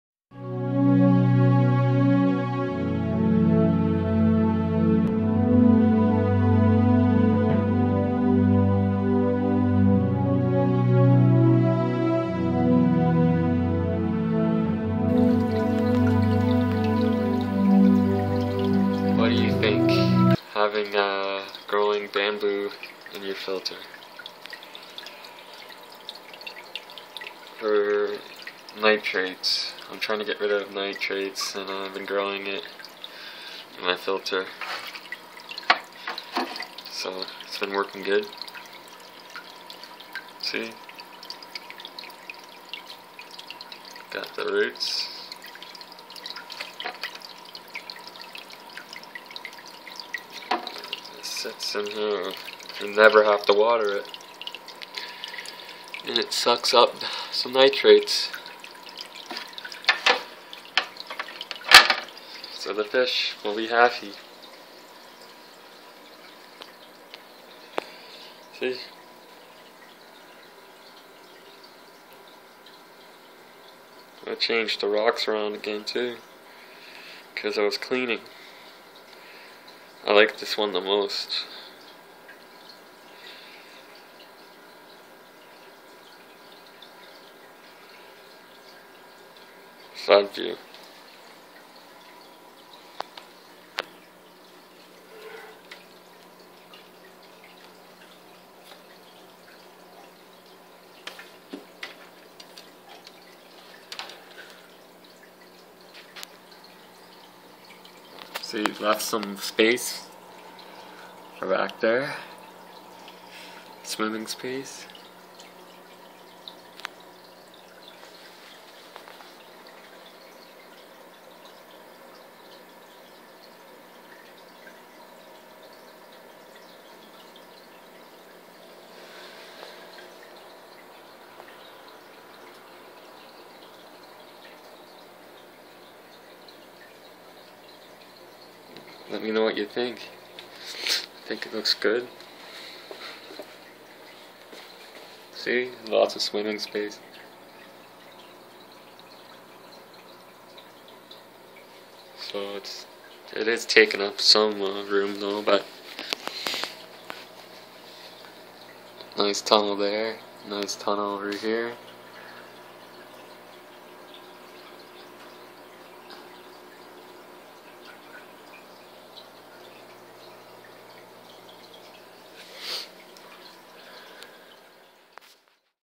Bamboo Growing in Filter
Growing bamboo to help with NITRATES in my fish tank